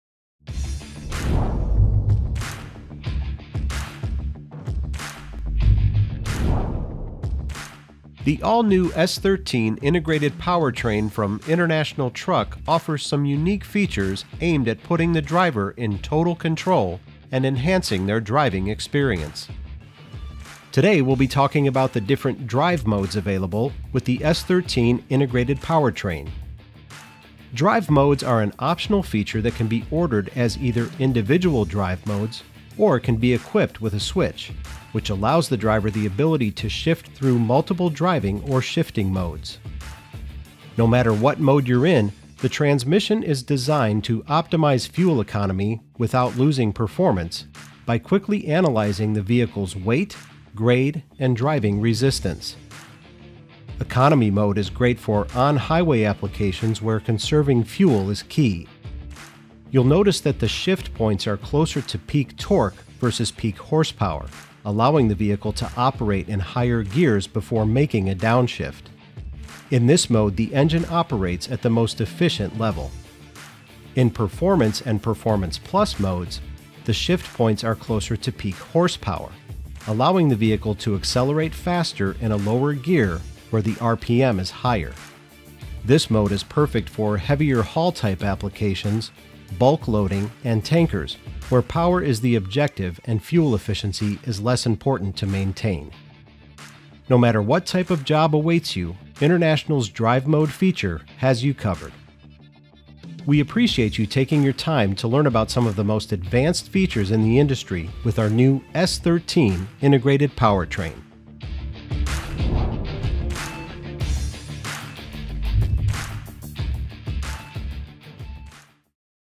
Drive Modes With International® S13 Integrated Powertrain
Great for on-highway applications the drive mode offered with the International® S13 Integrated Powertrain allows the driver to shift through multiple driving or shifting modes while ensuring no matter what mode they are in the transmission optimizes fuel economy and performance. Shift What's Possible with the new S13 Integrated Powertrain.